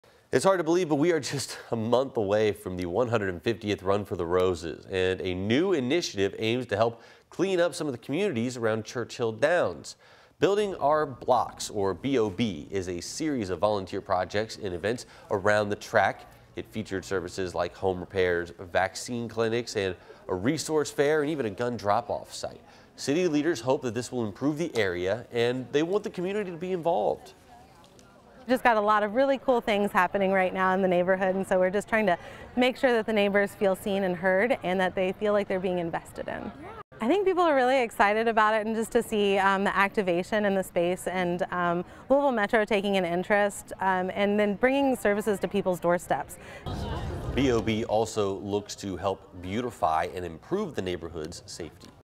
'Building Our Blocks' initiative to improve neighborhoods around Churchill Downs
The series of volunteer projects featured services like home repairs, vaccine clinics and a resource fair.